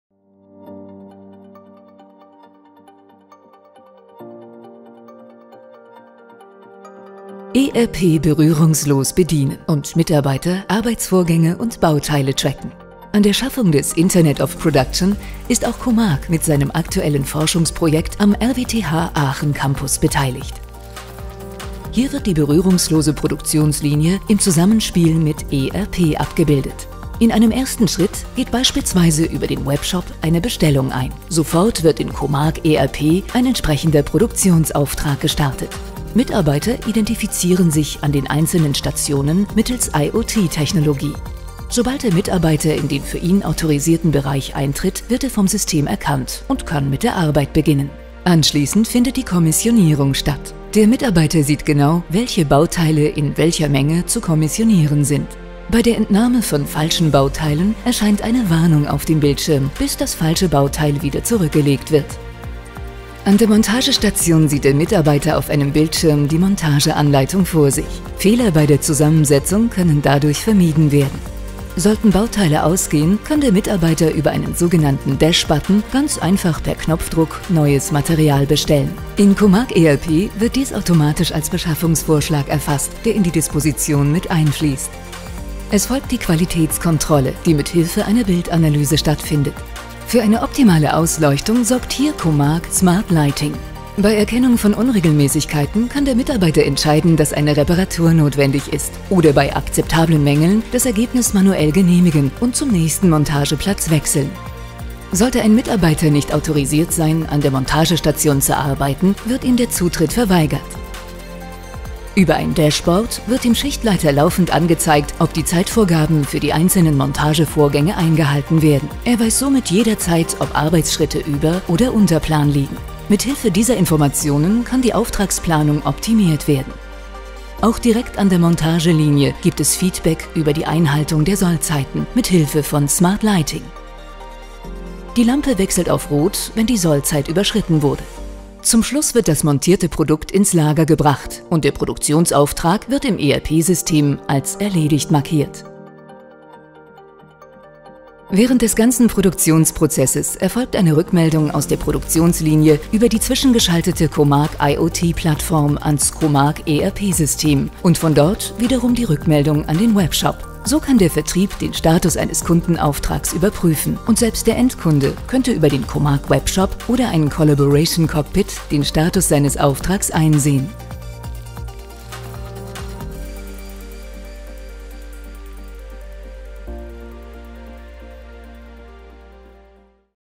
Digitalisierung der Fertigung mit Comarch: Abbildung der berührungslosen Produktionslinie
Comarch beteiligt sich seit Anfang 2018 mit Comarch ERP und Comarch IoT als Forschungspartner im Center Connected Industry und Center Enterprise Resource Planing auf dem RWTH Aachen Campus. Im Centerprojekt RTLS 3.0 wird das Comarch ERP-System mit einem Real-Time-Location-System (RTLS) so gekoppelt, dass Sensordaten Rückmeldeinformationen im ERP-System erzeugen.